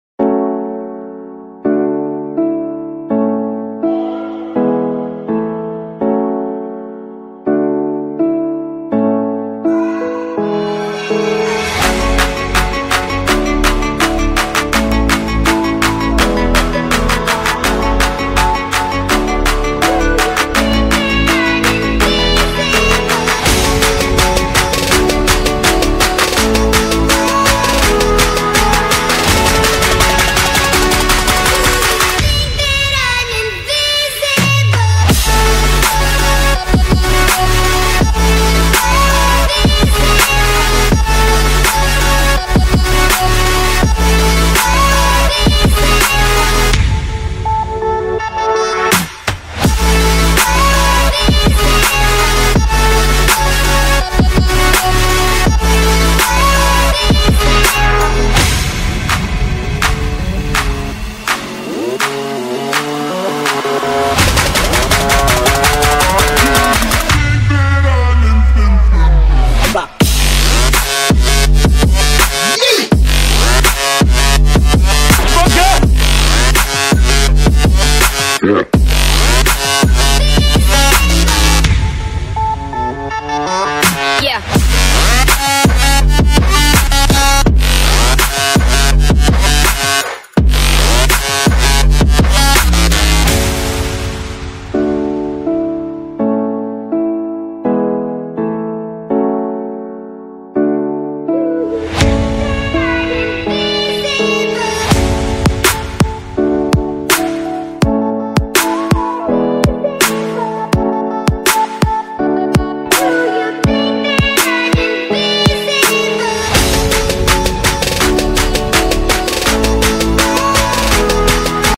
The 6 best exercises for the forearm muscles in bodybuilding - show the veins in the forearms quickl
The best forearm exercises to show veins